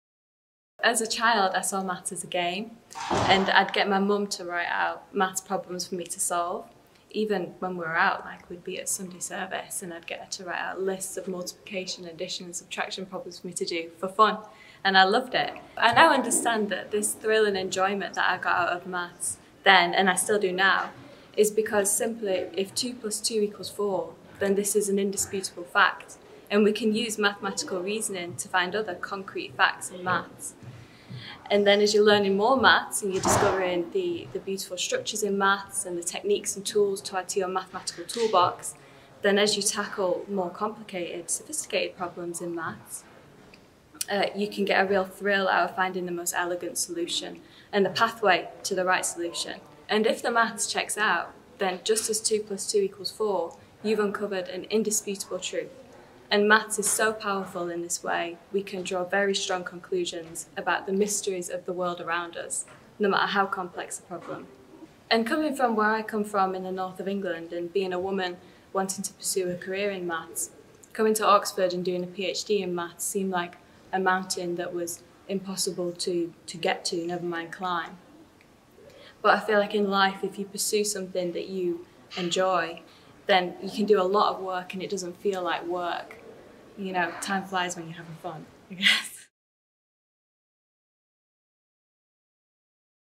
When did you know it was Maths? - Georgia
2 + 2 = 4

Mathematics as indisputable truth. Manchester's finest, Georgia Brennan, describes what made her a mathematician.

Episode 4 of 'When did you know it was Maths?' Stories of epiphanies and gradual dawns.